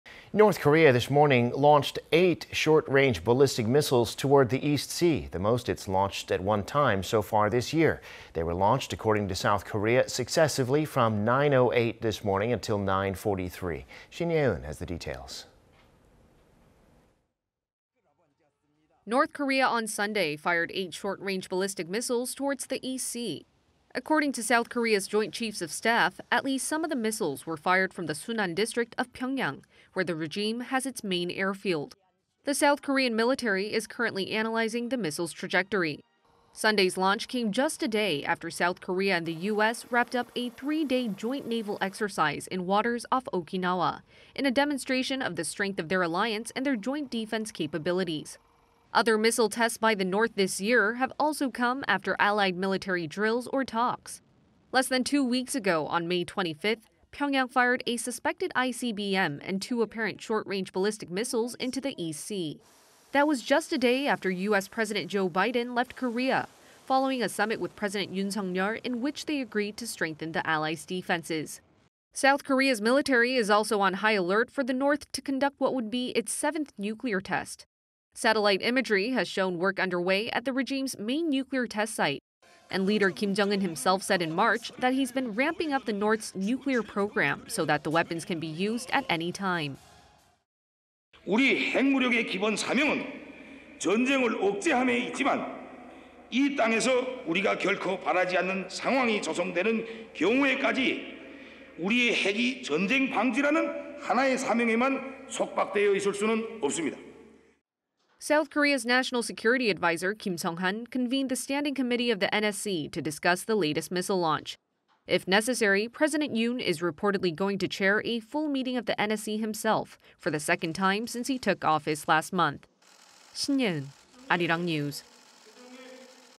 N. Korea launches 8 short-range ballistic missiles on Sunday
북, 동해상으로 탄도미사일 발사...올해 18번째 무력시위: 합참

North Korea this morning launched eight short-range ballistic missiles toward the East Sea -- the most it's launched at one time so far this year.
They were launched, according to South Korea, successively from 9:08 this morning until 9:43.
Shin Ye-eun has the details.
North Korea on Sunday fired eight short-range ballistic missiles towards the East Sea.
According to South Korea's Joint Chiefs of Staff, at least some of the missiles were fired
from the Sunan District of Pyeongyang, where the regime has its main airfield.
The South Korean military is currently analyzing the missiles' trajectory.

Sunday's launch came just a day after South Korea and the U.S. wrapped up a three-day joint naval exercise in waters off Okinawa... in a demonstration of the strength of their alliance and their joint defense capabilities.

Other missile tests by the North this year have also come after allied military drills or talks.
Less than two weeks ago, on May 25th,... Pyeongyang fired a suspected ICBM and two apparent short-range ballistic missiles into the East Sea.
That was just a day after U.S. President Joe Biden left Korea... following a summit with President Yoon Suk-yeol in which they agreed to strengthen the allies' defenses.

South Korea's military is also on high alert for the North to conduct what would be its seventh nuclear test.
Satellite imagery has shown work underway at the regime's main nuclear test site.
And leader Kim Jong-un himself said in March that he's been ramping up the North's nuclear program so that the weapons can be used at any time.

"We've been building our nuclear capability to prevent war. But, if we are put in a situation we don't desire, we may have to use it."

South Korea's National Security Adviser Kim Sung-han convened the standing committee of the NSC to discuss the latest missile launch.
If necessary, President Yoon is reportedly going to chair a full meeting of the NSC himself... for the second time since he took office last month.
Shin Ye-eun, Arirang News.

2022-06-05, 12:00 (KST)